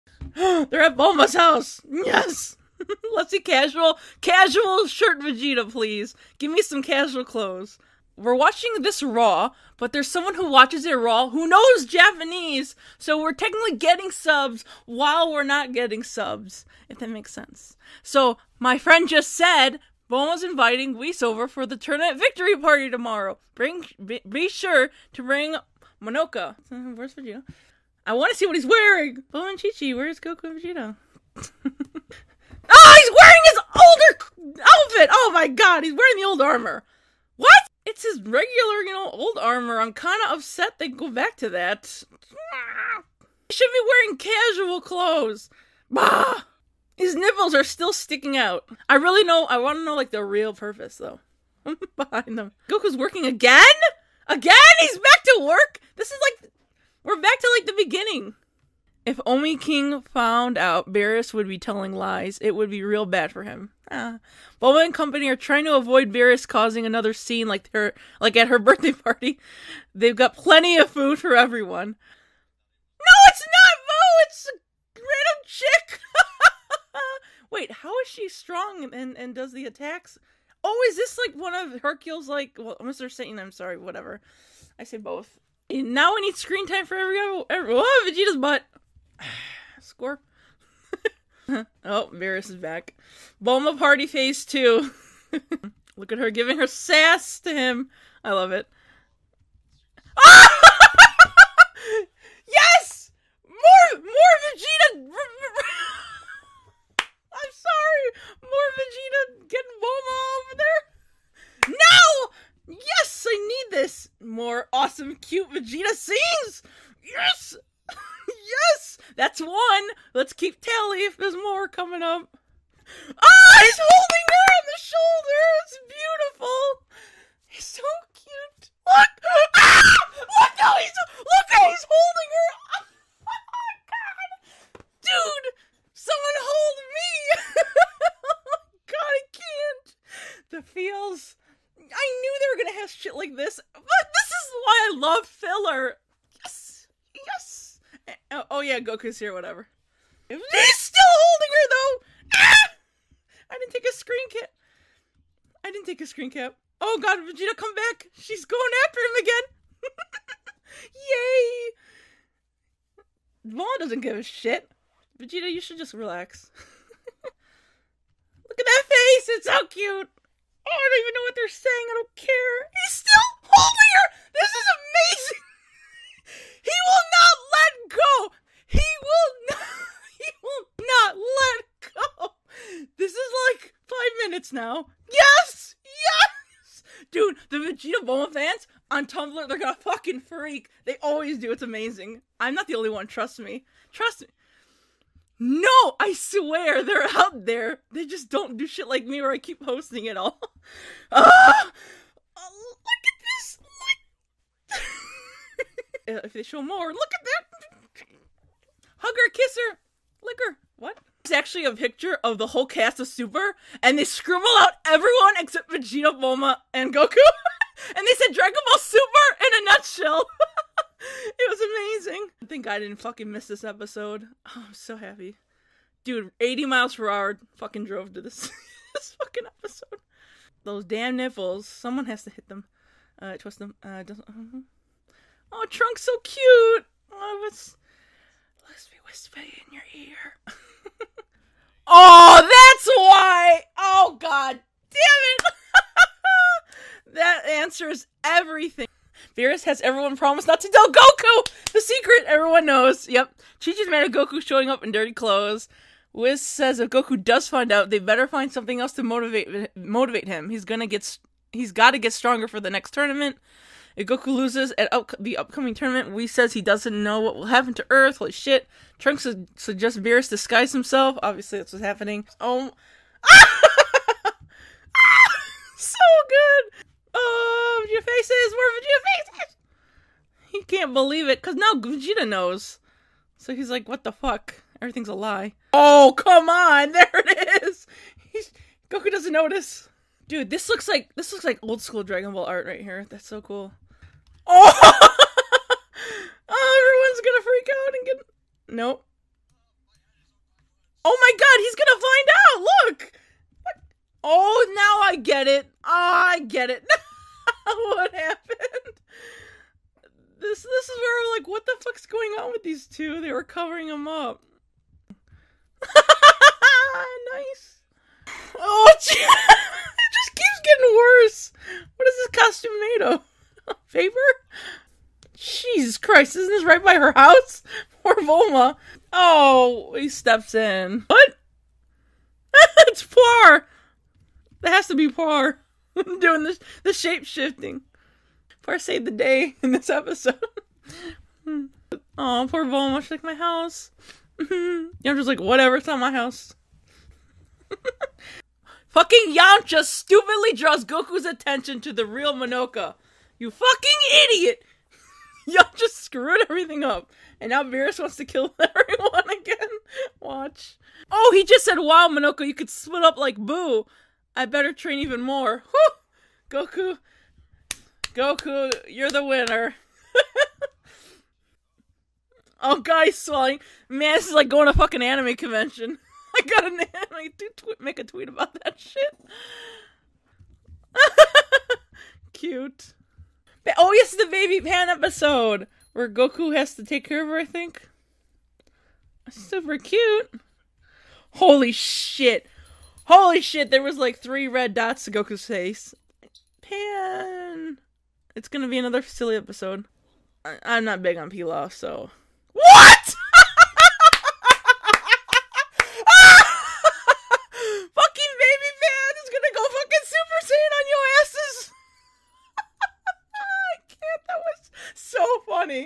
MsDBZbabe reaction to Dragon Ball Super Episode 42
Enjoy my reaction to Dragon Ball Super Episode 42

I GOOFED UP, I only recorded my audio & not the Skype convo, sadly this video will only feature me talking, shit happens, but I was screaming 90% of the time anyways, sorry!

LOWER the volume!!